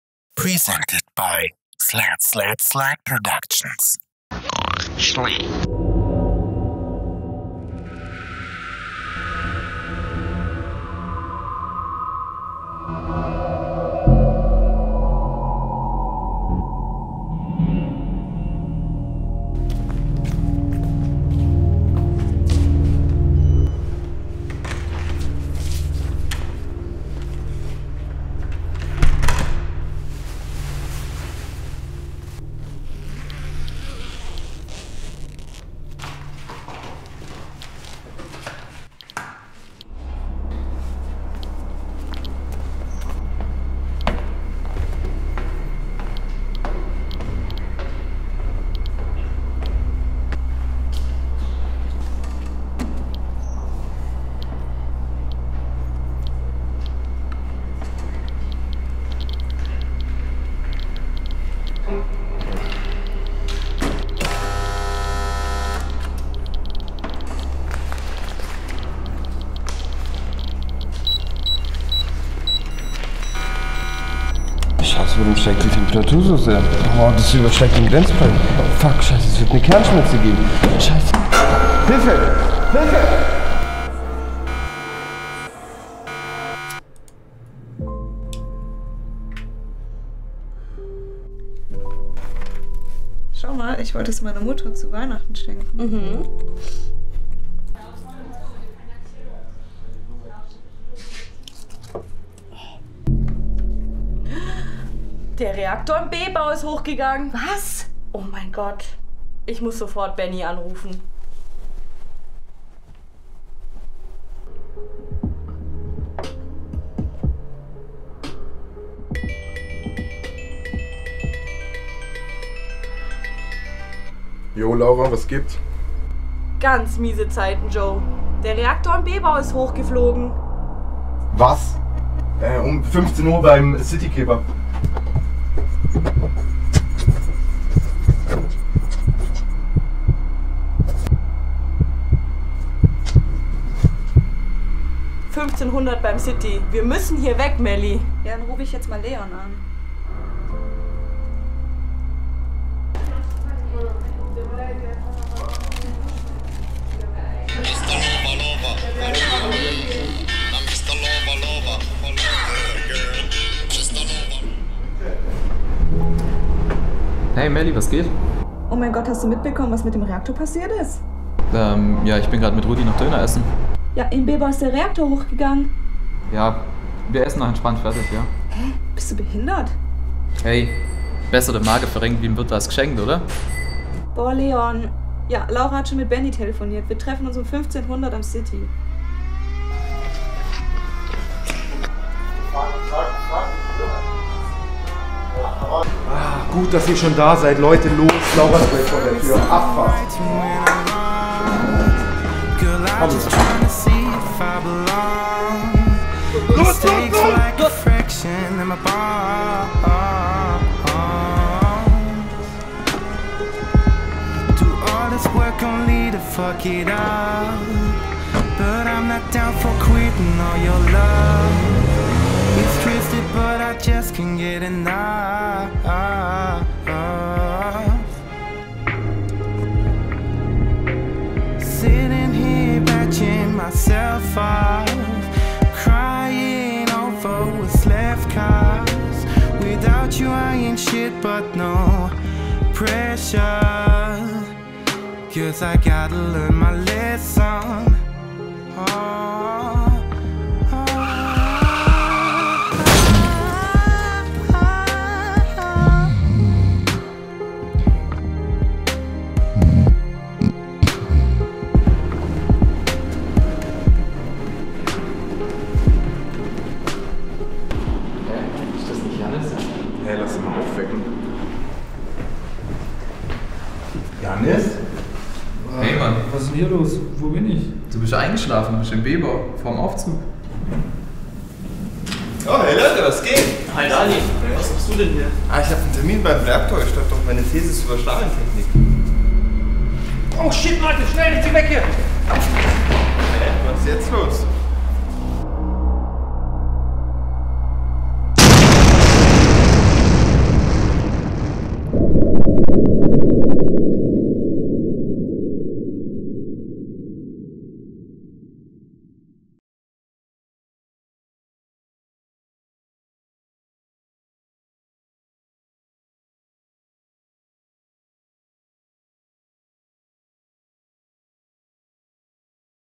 Fallout Furti
Ein Kurzfilm, der als Semesterprojekt im 2. Semester Online Medien, im Rahmen der Veranstaltung AV-Produktion, an der Hochschule Furtwangen entstanden ist.

Regie: Laura Greim, Benjamin Slaneinski, Leon Schäfer
Kamera: Laura Greim, Benjamin Slaneinski, Leon Schäfer, Dennis Strizek
Ton: Redouane Ainouz, Jannis Reichel, Benjamin Slaneinski
Intro: Benjamin Slaneinski, Leon Schäfer
Licht: Benjamin Slaneinski
Schnitt: Melanie Fleischer, Laura Greim, Benjamin Slaneinski, Leon Schäfer, Redouane Ainouz
Blender Atompilz Animation: Jannis Reichel
Musik & Filmvertonung: Benjamin Slaneinski, Leon Schäfer
Mastering: Benjamin Slaneinski, Laura Greim

Besonderes Dankeschön an:

Daniel Schoppa
Herr Hussain
Herr Costa
Dennis Strizek
Alessa Baldischwieler

Betreuer: Herrn Prof. Aichele und Herrn Prof. Reusch
Wintersemester 2019/2020

Sounds: Soundlibary Hochschule Furtwangen